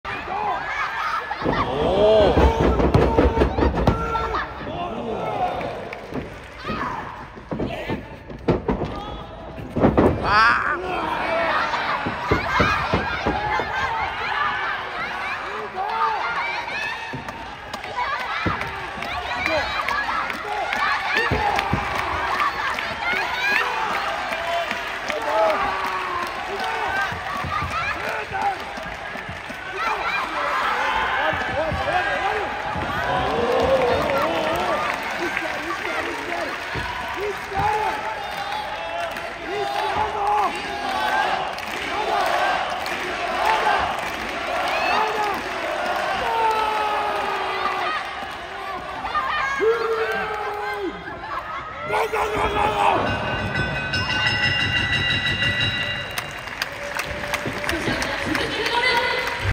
鈴木みのるをなんとか伊藤麻希が丸め込もうとするもみのるの鋭い動きでバックに回られるとスリーパーで締め上げられ、なんとか粘るも… 東京女子プロレス「GRAND PRINCESS '24」
東京女子プロレス「GRAND PRINCESS '24」 2023年3月31日

第七試合　20分一本勝負
スペシャル・シングルマッチ
鈴木みのる VS 伊藤麻希

・動画撮影・公開
(2022/04/14確認)

・東京女子プロレスの公式動画は下記で見ることができます！